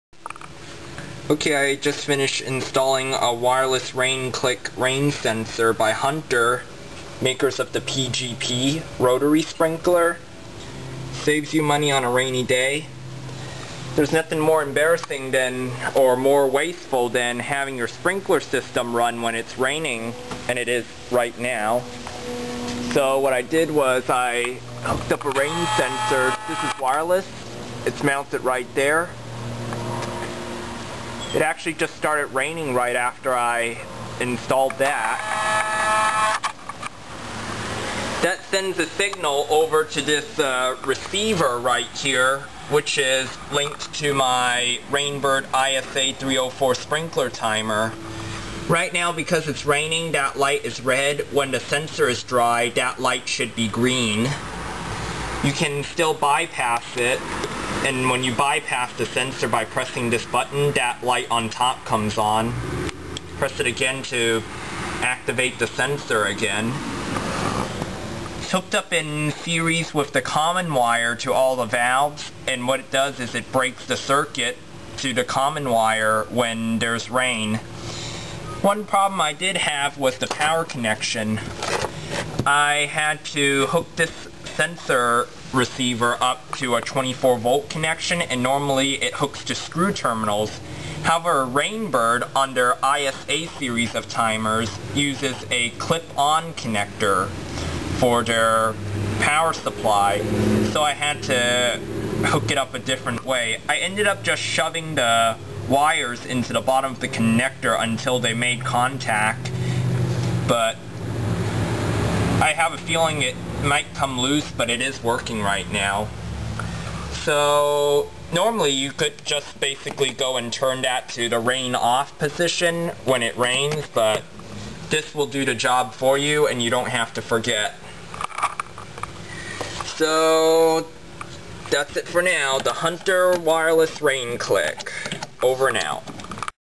Hunter Wireless Rain-Clik rain sensor
I added a rain sensor to my sprinkler system. This is the wireless "Rain-Clik" rain sensor by Hunter with Quick Response technology which allows the sensor to detect rain quickly and shut the sprinkler system down almost instantly unlike competitive units which must wait for rain water to fill or soak up the unit before the switch is flipped.

Being a wireless unit helps with the install as no wires were needed between the controller and the sensor itself; just mount the receiver near the sprinkler timer, connect wires for power and control, and place the sensor/transmitter unit anywhere within 300 feet of the receiver where it will receive rainfall.

Just after mounting the sensor, it started to rain on me.